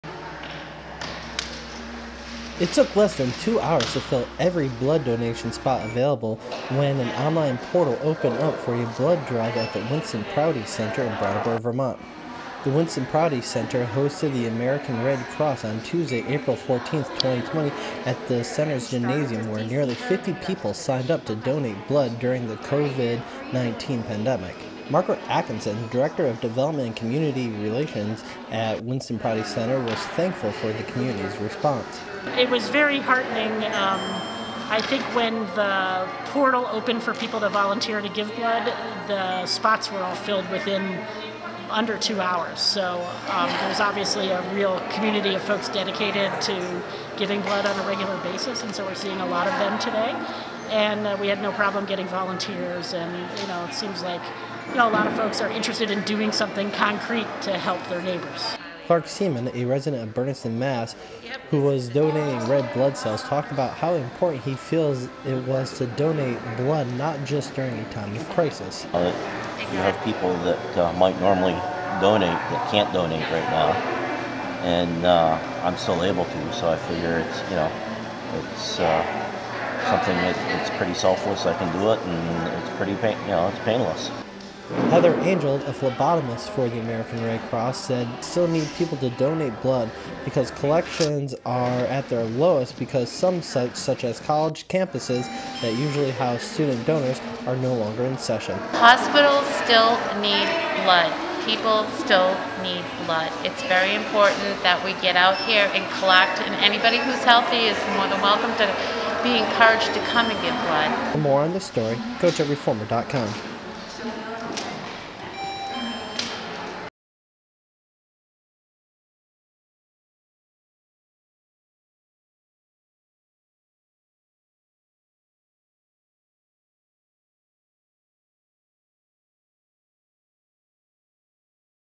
Donating blood
Winston Prouty Center, in Brattleboro, Vt., teamed up with the American Red Cross, from Manchester, N.H., to hold a blood drive at the gymnasium located on the campus on Tuesday, April 14, 2020.